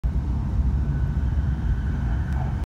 北陸新幹線の車窓①（2020年12月）
北陸新幹線で黒部宇奈月温泉駅を通過後の車窓から外景色です。
黒部川と遠くには北アルプスの山々を見えることが出来ます。